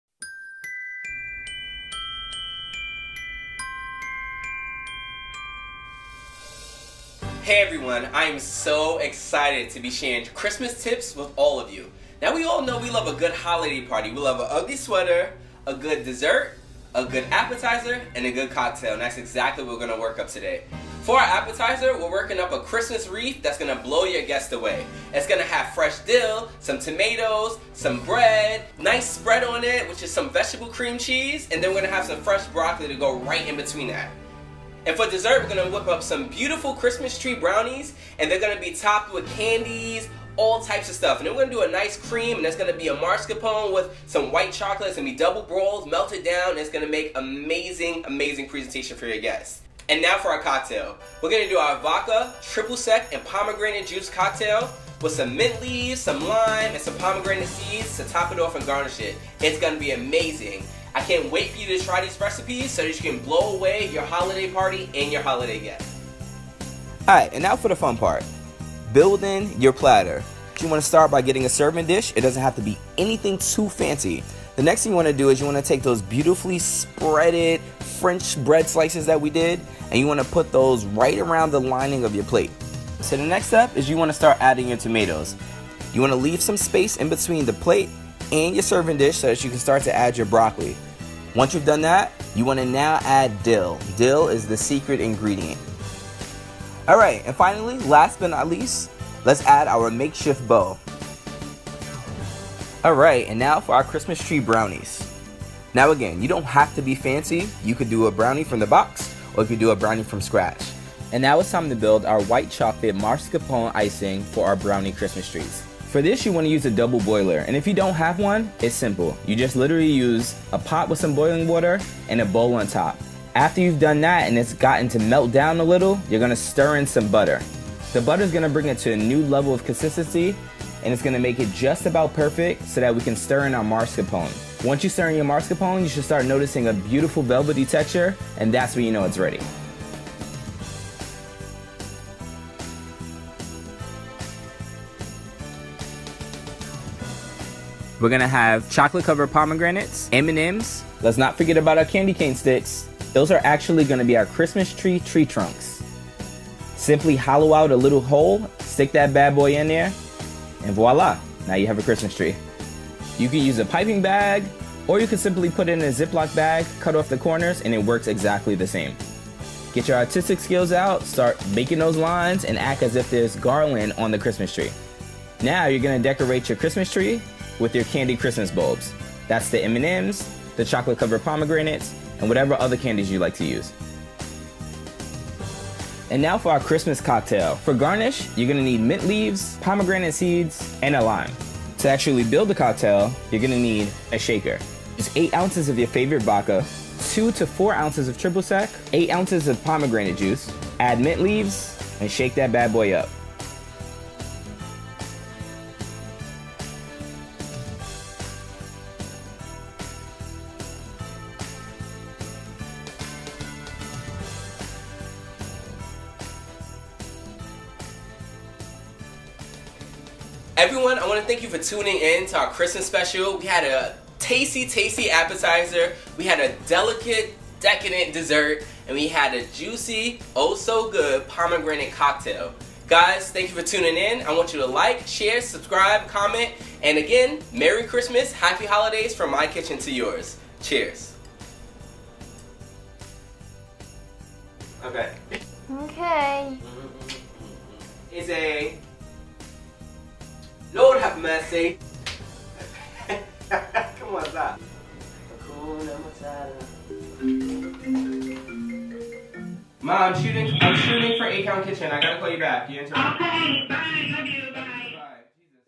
Holiday Reef Party Platter, Christmas Trees Brownies + Cocktail | 8 Count Kitchen Christmas Special:
Hey everyone! 8 Count Kitchen is back again and just in time for the holidays! Whether you're planning a Holiday Party,  attending one, or simply looking for a way to spice up your Christmas dinner, this episode is for you! Grab a pen, some paper, and get to writing because these recipes are going to blow your guest away! Merry Christmas and Happy Holidays, from my kitchen to yours!

Video Shot and Edited by: Linda Inthongpradith